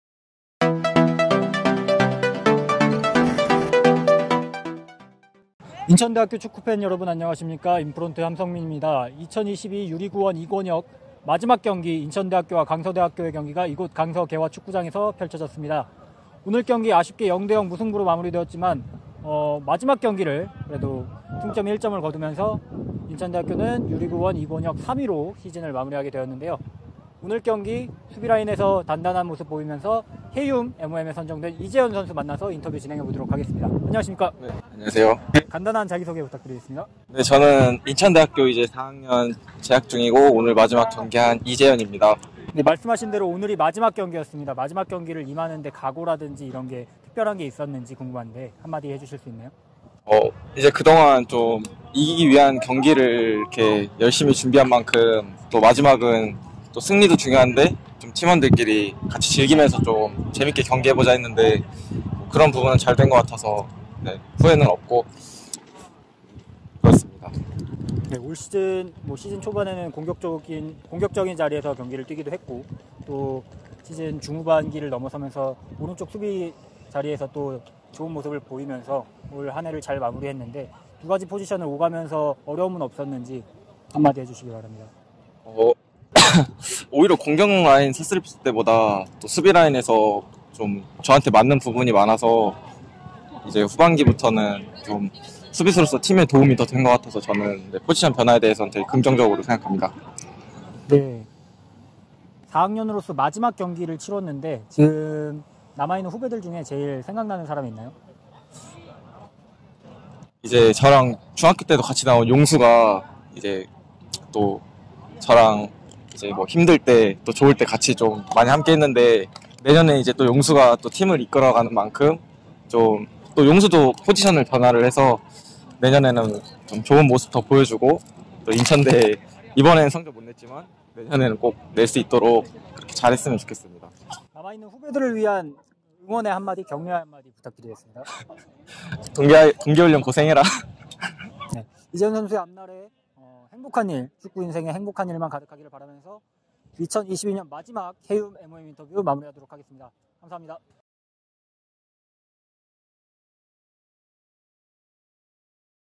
[ 혜윰 MOM 인터뷰🎤 ] 2022 U리그1 18R 강서대전 MOM 이재현 선수 인터뷰
[ 혜윰 MOM 인터뷰🎤 ]
2022 U - LEAGUE 1 2권역 18R
인천대학교 0 - 0 강서대학교

강서대전 MOM 이재현 선수의 인터뷰를 영상으로 만나보세요!